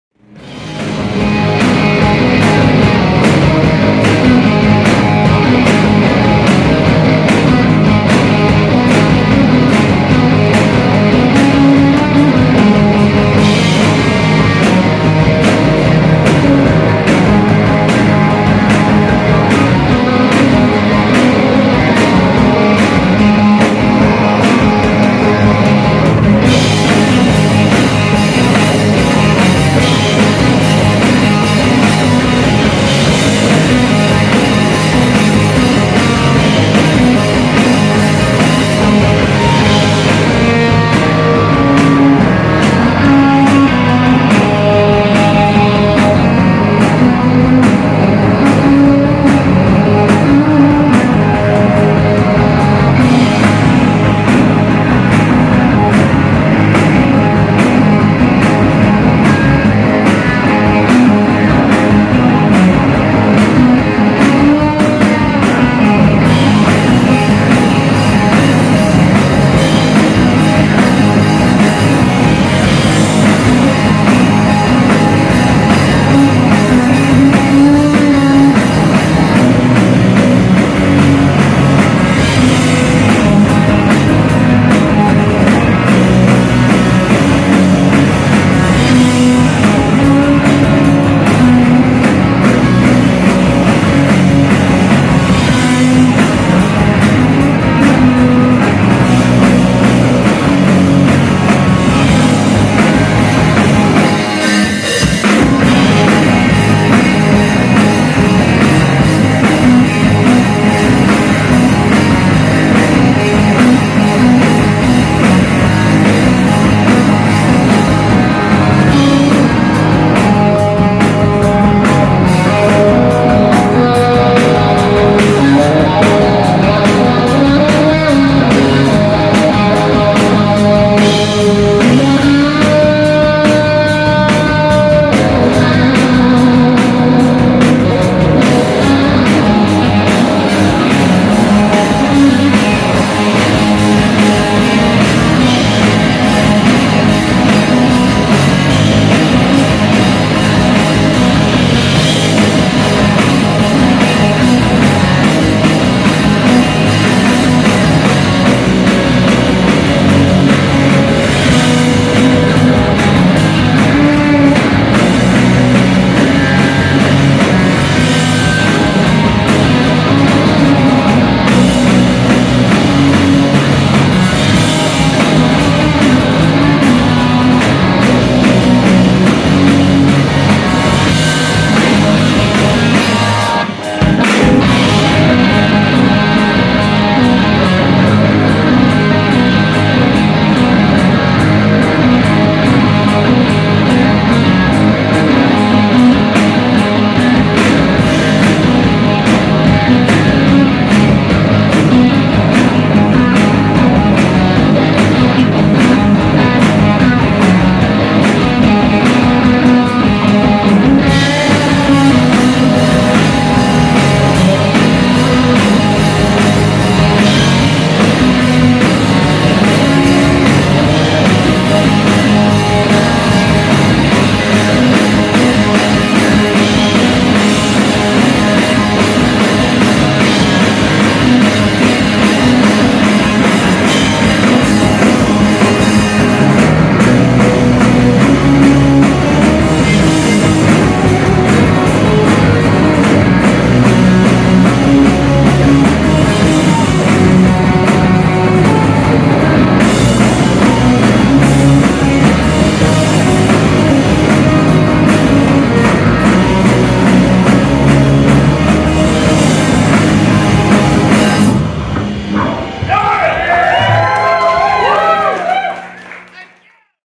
Severed Satellites @ Olde Mothers Brewery in Frederick, Maryland
Severed Satellites @ Olde Mothers Brewery in Frederick, Maryland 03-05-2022